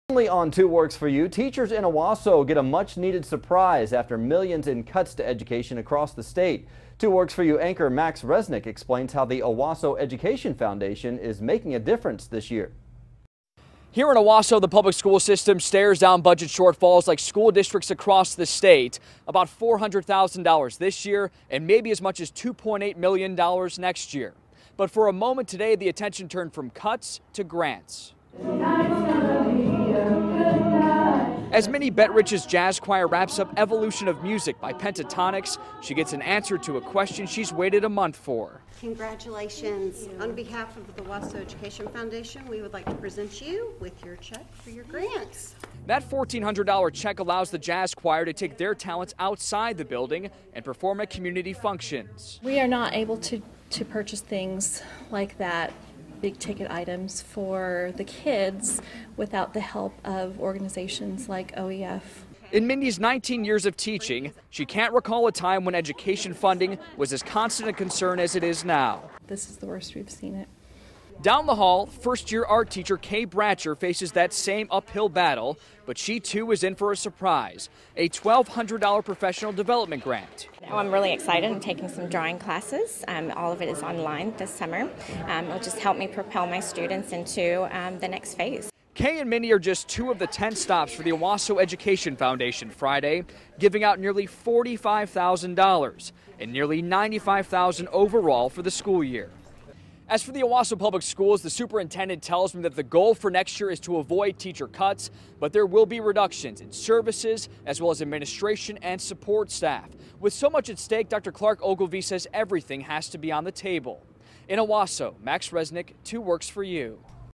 Owasso Teachers Awarded Grant Money
Owasso Education Foundation Grants Thousands Of Dollars